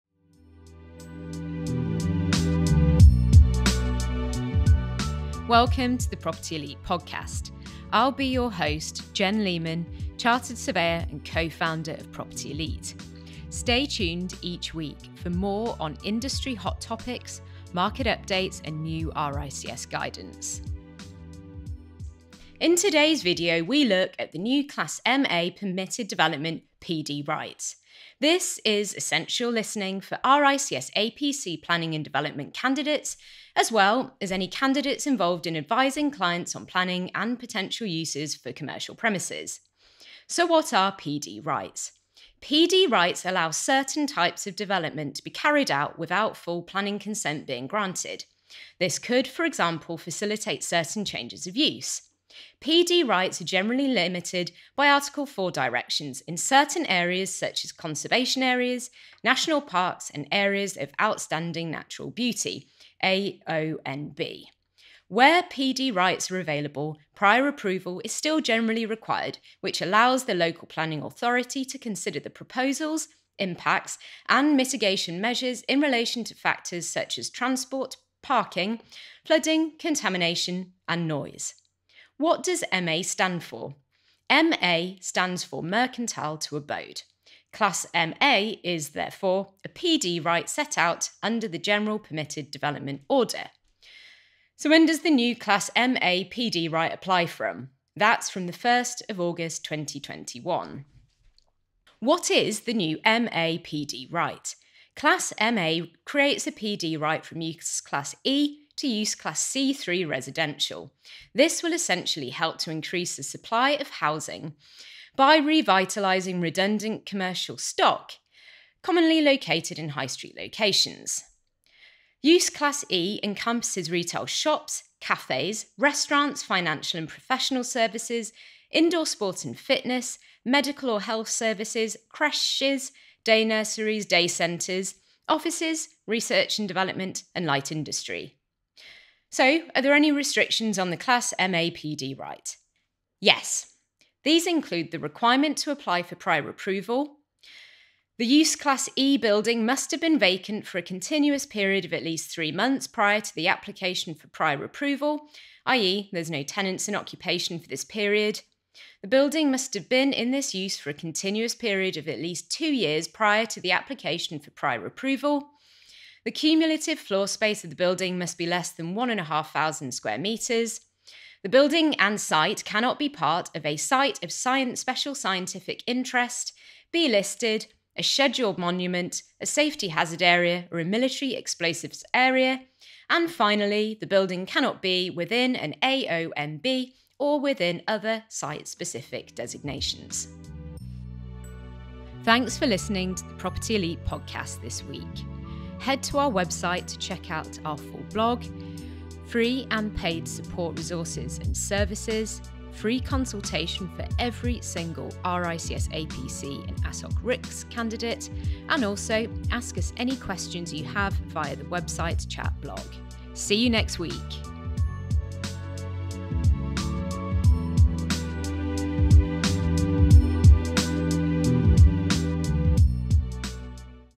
Class MA Permitted Development Right - Hot Topic Highlight
In this week's video, we look at the new Class MA permitted development (PD) right.

This is essential reading for RICS APC Planning & Development candidates, as well as any candidates involved in advising clients on planning and potential uses for commercial premises.